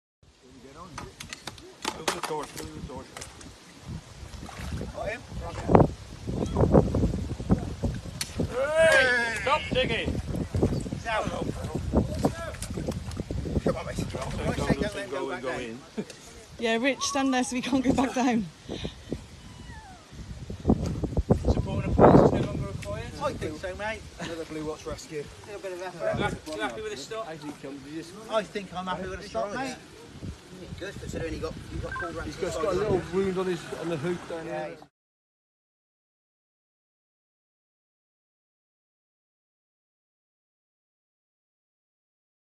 Firemen rescue adorable lamb stuck in a tunnel in a field
The sheep was rescued from a storm drain in Belmont, Herefordshire, on Tuesday. It had taken a dip in the drain on a hot day before it became stuck inside. Firefighters rescued the sheep with a telescopic shepherd's crook at 1.20pm in heartwarming footage.